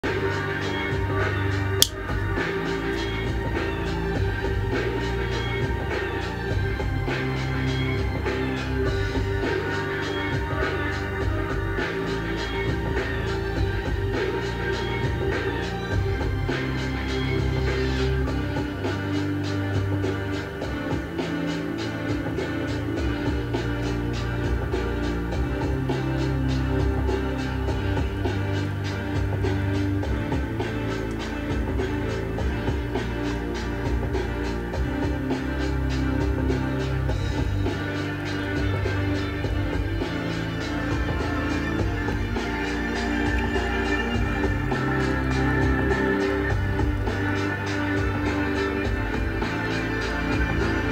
E-MTB informační jednotka s AVR
vývoj nové informační a řídící jednotky na elektrický mountainboard, ve finální verzi zobrazení napětí, proudu, aktuálního výkonu, odebrané energie z baterie, otáčky, rychlost, vzdálenost, čas, teplota,.